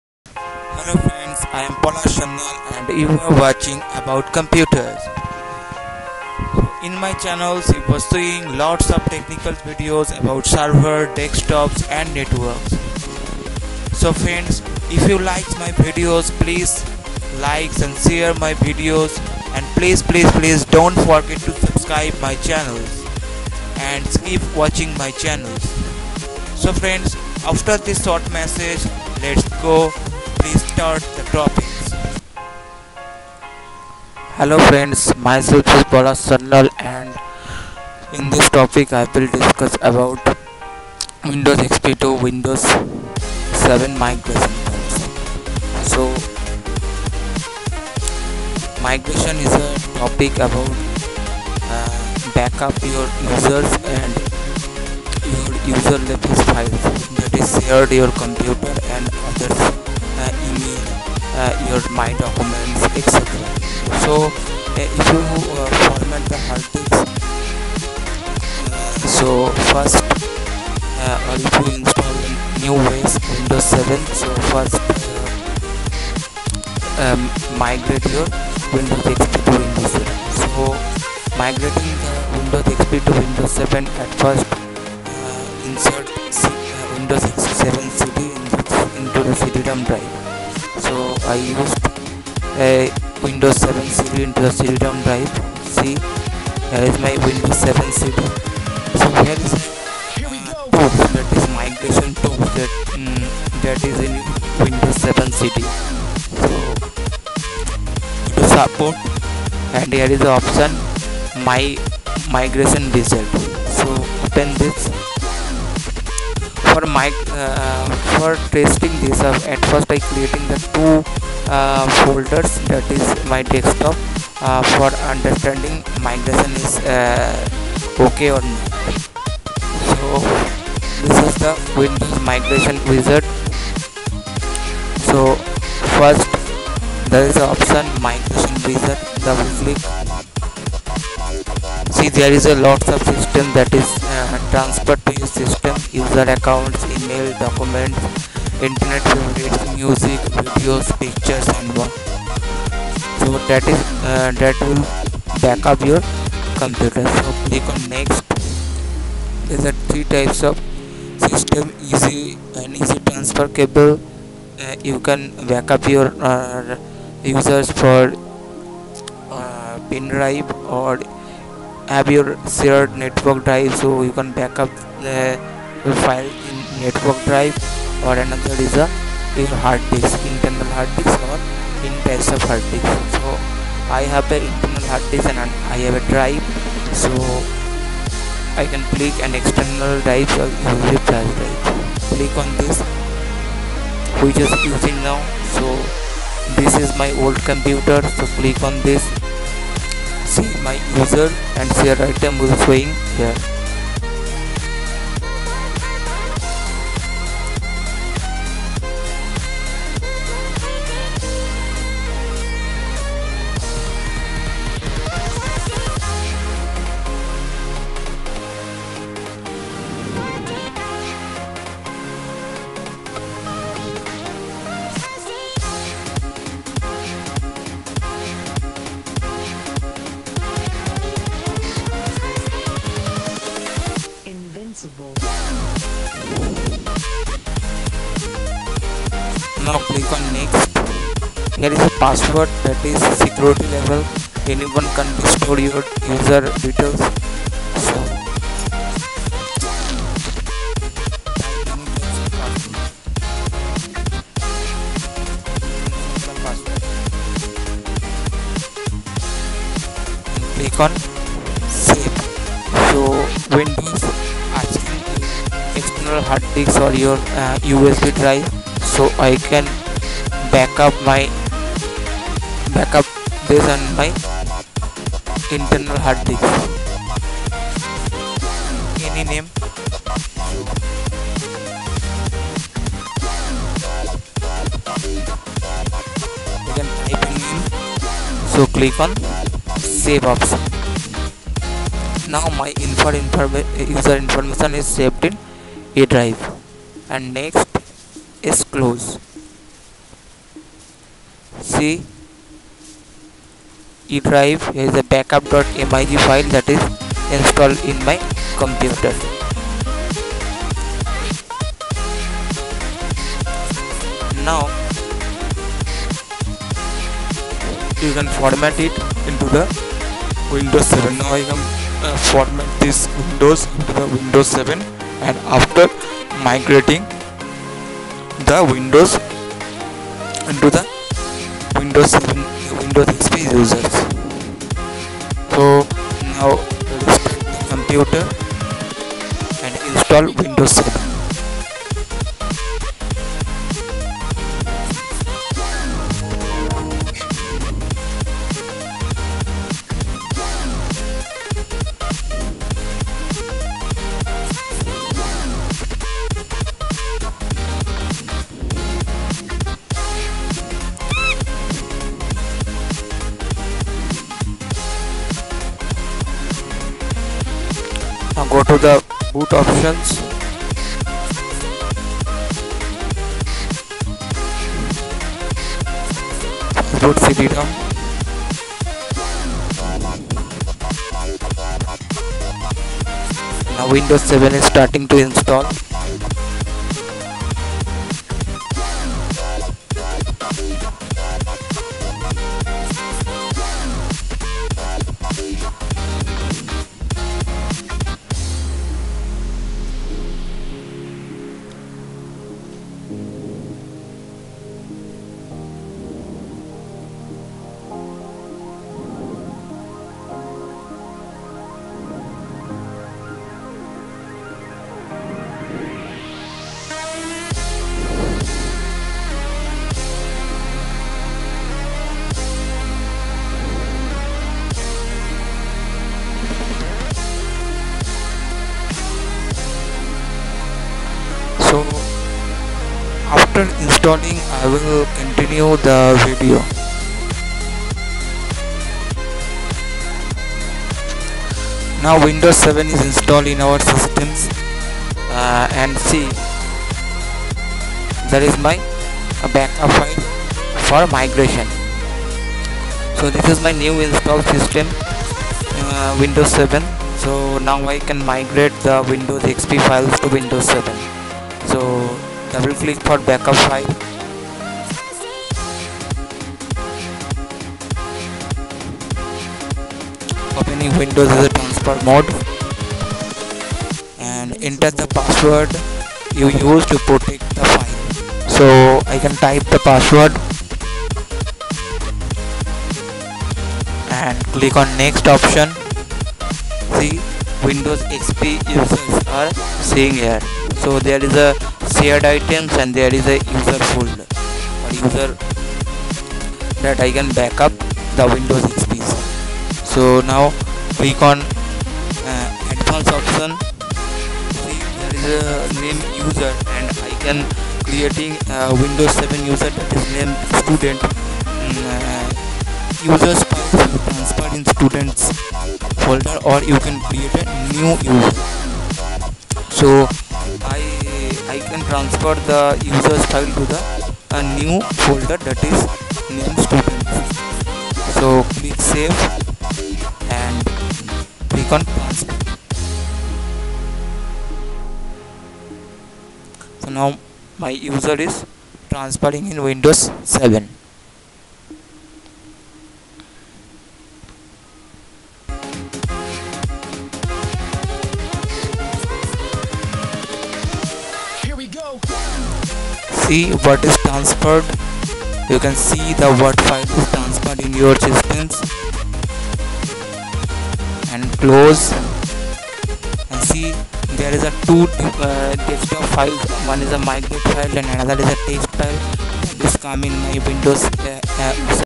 How to Migrate or upgrade windows XP to windows 7,8,10 ?
hello friends in this video I show you how to migrate your windows xp machine to windows 7 . using migration you can backup your user profile your e-mail accounts , your documents picture etc, this is very useful if you format a machine . you can do it to migrate windows xp to other os like windows 8,10 , . the process is same for all types of os.if any doubt please comment. or you can mail me